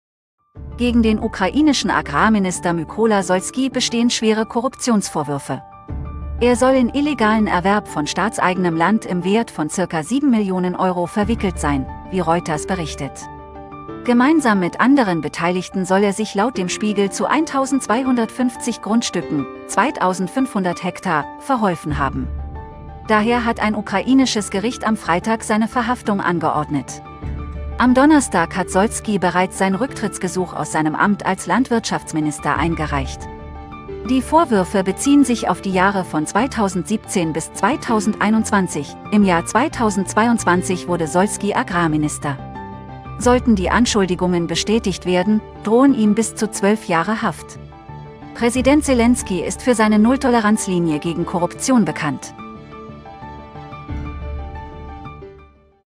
Korruptionsskandal in der Ukraine: Agrarminister Solskyj (44) verhaftet!
Gegen den ukrainischen Agrarminister Mykola Solskyj bestehen schwere Korruptionsvorwürfe. Er soll in illegalen Erwerb von staatseigenem Land im Wert von circa 7 Millionen Euro verwickelt sein, wie "Reuters" berichtet.